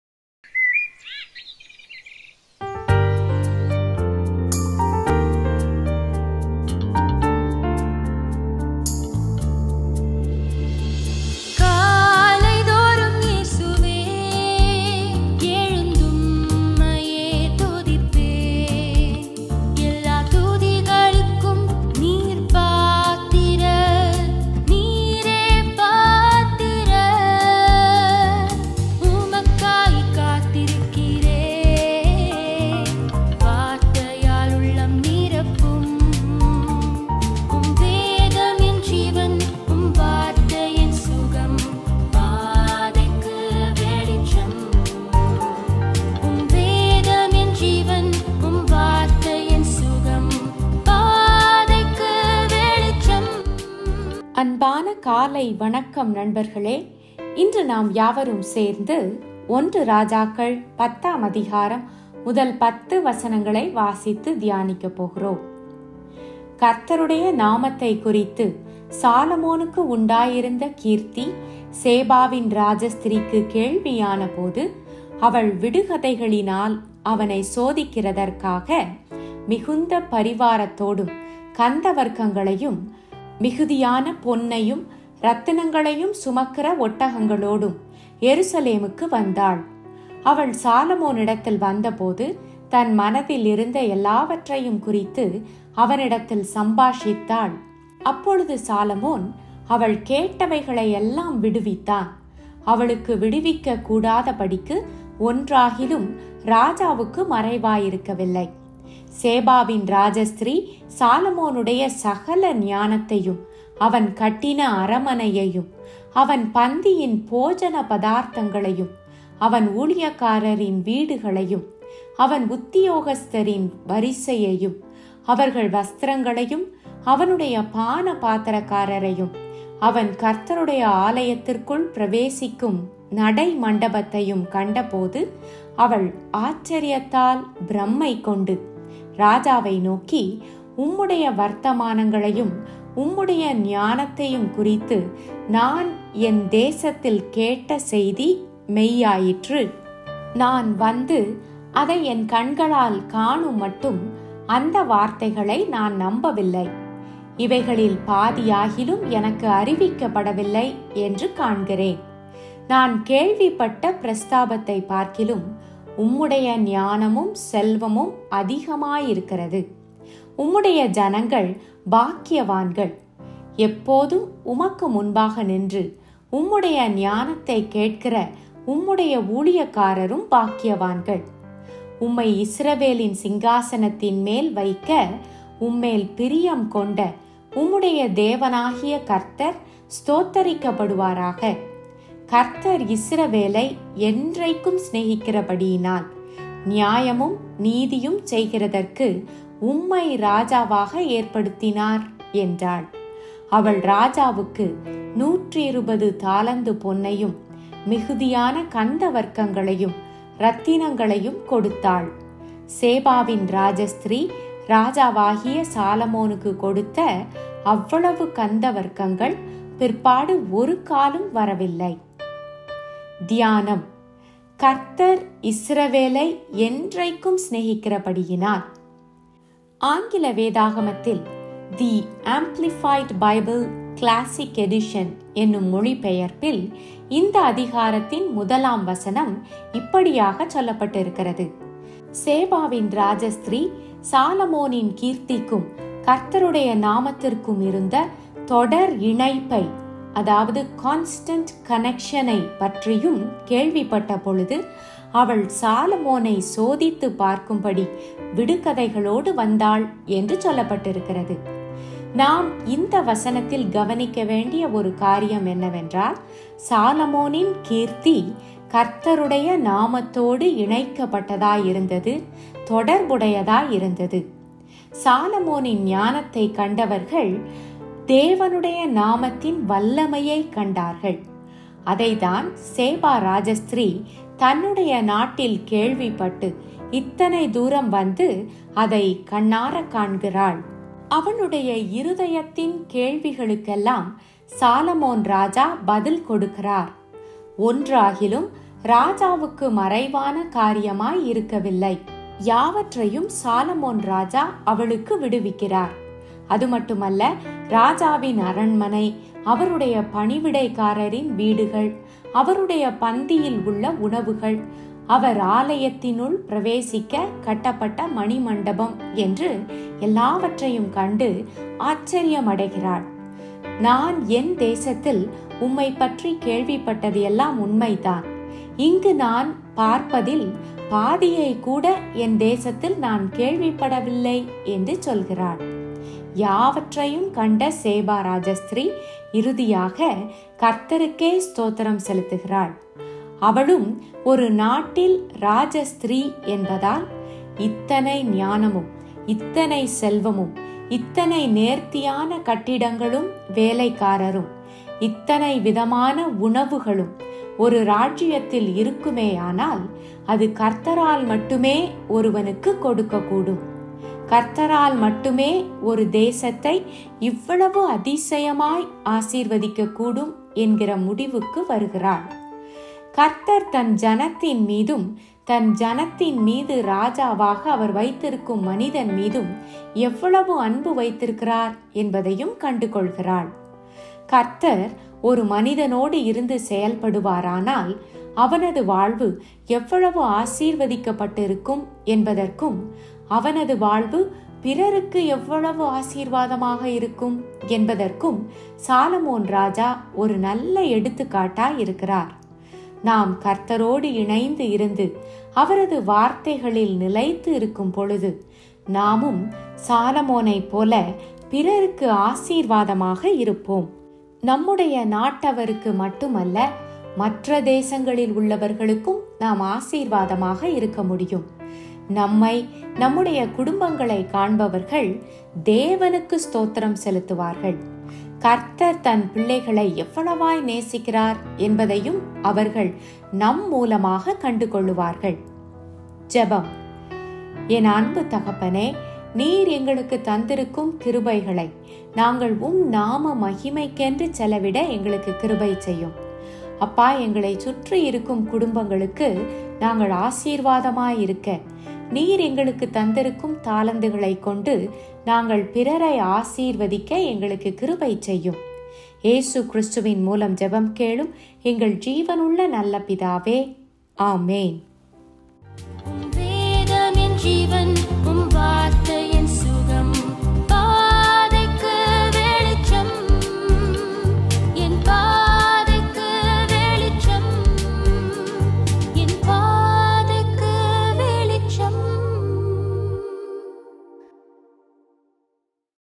கர்த்தர் இஸ்ரவேலை என்றைக்கும் சிநேகிக்கிறபடியினால்...
1 ராஜாக்கள் : 10 : 1-10
1. கர்த்தருடைய நாமத்தைக்குறித்துச் சாலொமோனுக்கு உண்டாயிருந்த கீர்த்தி சேபாவின் ராஜஸ்திரீக்குக் கேள்வியானபோது, அவள் விடுகதைகளினால் அவனைச் சோதிக்கிறதற்காக,

2. மிகுந்த பரிவாரத்தோடும், கந்தவர்க்கங்களையும், மிகுதியான பொன்னையும் இரத்தினங்களையும் சுமக்கிற ஒட்டகங்களோடும், எருசலேமுக்கு வந்தாள்; அவள் சாலொமோனிடத்தில் வந்தபோது, தன் மனதில் இருந்த எல்லாவற்றையுங்குறித்து அவனிடத்தில் சம்பாஷித்தாள்.

3. அப்பொழுது சாலொமோன் அவள் கேட்டவைகளையெல்லாம் விடுவித்தான், அவளுக்கு விடுவிக்கக்கூடாதபடிக்கு, ஒன்றாகிலும் ராஜாவுக்கு மறைபொருளாயிருக்கவில்லை.

4. சேபாவின் ராஜஸ்திரீ சாலொமோனுடைய சகல ஞானத்தையும், அவன் கட்டின அரமனையையும்,

5. அவன் பந்தியின் போஜனபதார்த்தங்களையும், அவன் ஊழியக்காரரின் வீடுகளையும், அவன் உத்தியோகஸ்தரின் வரிசையையும், அவர்கள் வஸ்திரங்களையும், அவனுடைய பானபாத்திரக்காரரையும், அவன் கர்த்தருடைய ஆலயத்துக்குள் பிரவேசிக்கும் நடைமண்டபத்தையும் கண்டபோது அவள் ஆச்சரியத்தால் பிரமைகொண்டு,

6. ராஜாவை நோக்கி: உம்முடைய வர்த்தமானங்களையும் உம்முடைய ஞானத்தையும் குறித்து நான் என் தேசத்தில் கேட்ட செய்தி மெய்யாயிற்று.

7. நான் வந்து அதை என் கண்களால் காணுமட்டும் அந்த வார்த்தைகளை நான் நம்பவில்லை; இவைகளில் பாதியாகிலும் எனக்கு அறிவிக்கப்படவில்லை என்று காண்கிறேன்; நான் கேள்விப்பட்ட பிரஸ்தாபத்தைப்பார்க்கிலும், உம்முடைய ஞானமும் செல்வமும் அதிகமாயிருக்கிறது.

8. உம்முடைய ஜனங்கள் பாக்கியவான்கள்; எப்போதும் உமக்கு முன்பாக நின்று, உம்முடைய ஞானத்தைக் கேட்கிற உம்முடைய ஊழியக்காரரும் பாக்கியவான்கள்.

9. உம்மை இஸ்ரவேலின் சிங்காசனத்தின்மேல் வைக்க, உம்மேல் பிரியங்கொண்ட உம்முடைய தேவனாகிய கர்த்தர் ஸ்தோத்திரிக்கப்படுவாராக: கர்த்தர் இஸ்ரவேலை என்றைக்கும் சிநேகிக்கிறபடியினால், நியாயமும் நீதியும் செய்கிறதற்கு உம்மை ராஜாவாக ஏற்படுத்தினார் என்றாள்.

10. அவள் ராஜாவுக்கு நூற்றிருபது தாலந்து பொன்னையும், மிகுதியான கந்தவர்க்கங்களையும், இரத்தினங்களையும் கொடுத்தாள்; சேபாவின் ராஜஸ்திரீ ராஜாவாகிய சாலொமோனுக்குக் கொடுத்த அவ்வளவு கந்தவர்க்கங்கள் பிற்பாடு ஒருக்காலும் வரவில்லை.